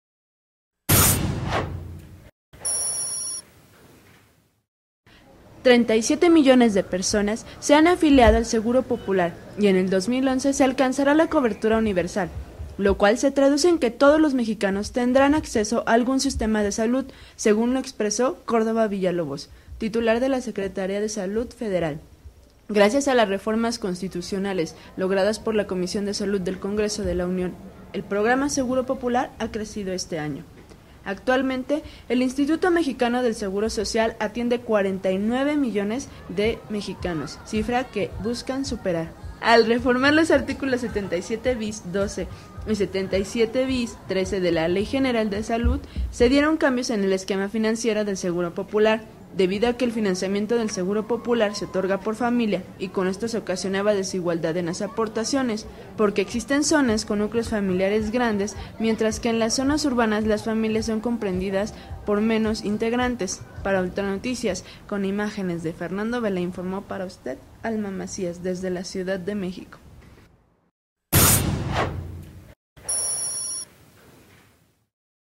Ya van 37 millones de personas inscritas al Seguro Popular
DF, 06 de Julio 2010.- Sinopsis.- Hasta el momento, 37 millones de personas se han afiliado al Seguro Popular, se estima que para el a&ntilde;o 2011 se alcanzar&aacute; la cobertura universal, lo cual se traduce en que todos los mexicanos tendr&aacute;n acceso a alg&uacute;n sistema de salud, seg&uacute;n lo expreso Jos&eacute; &Aacute;ngel C&oacute;rdova Villalobos, titular de la Secretaria de Salud.